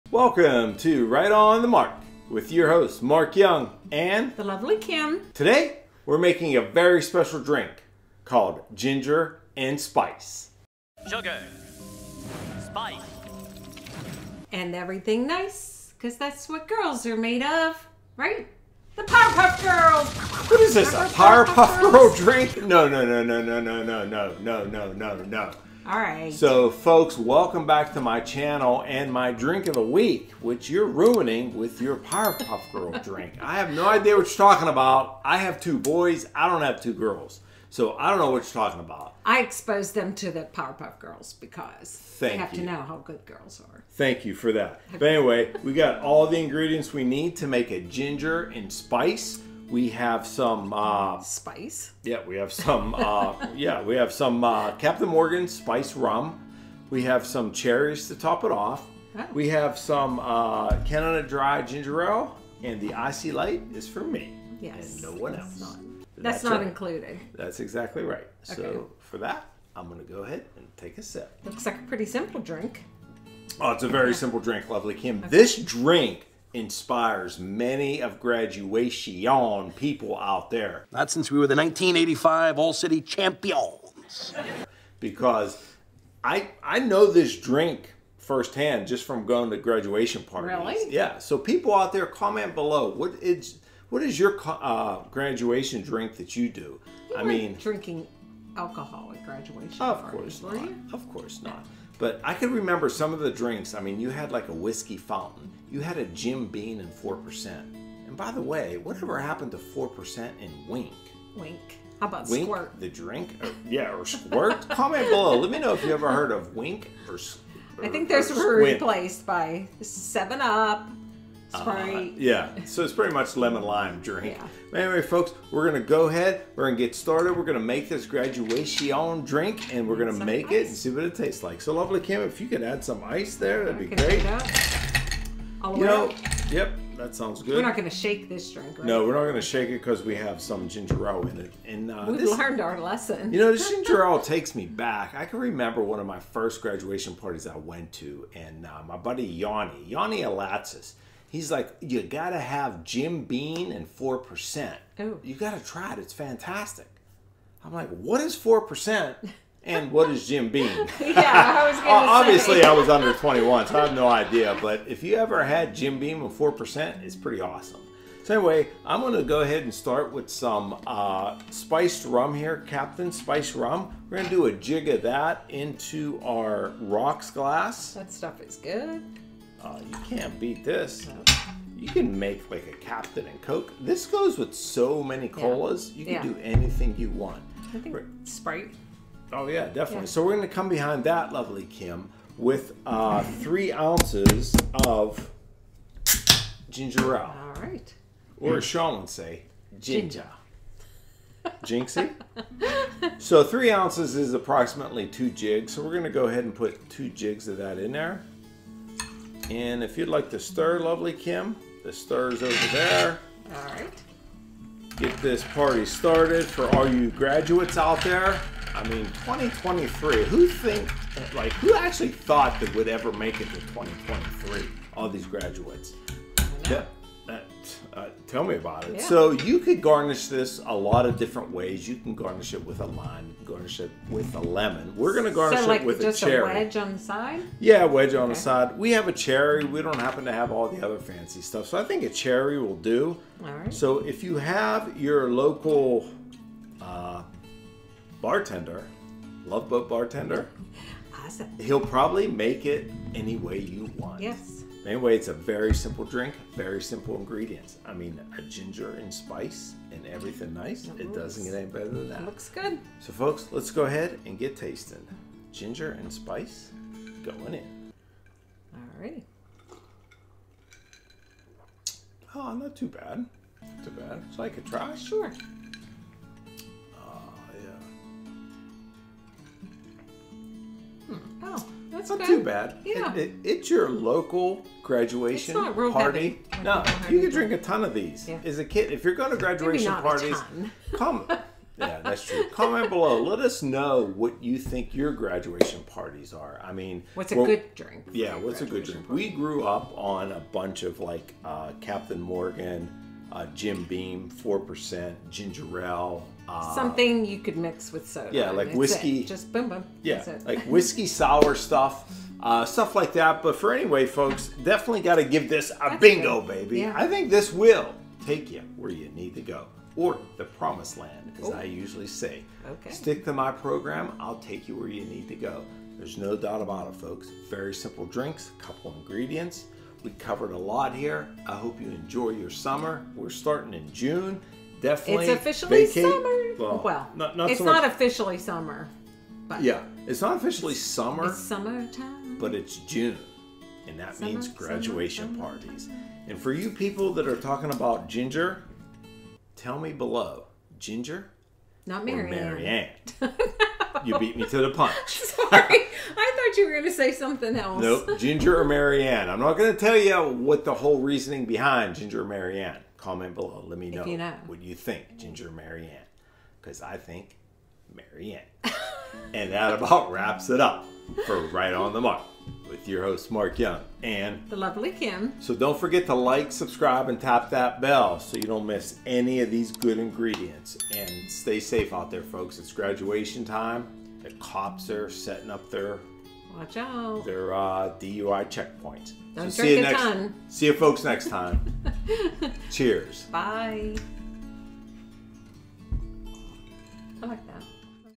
Ginger and Spice - Right On The Mark 234
Mark and The Lovely Kim make a Ginger and Spice in celebration of the new graduates of 2023! Aw Yeah! Bingo Baby!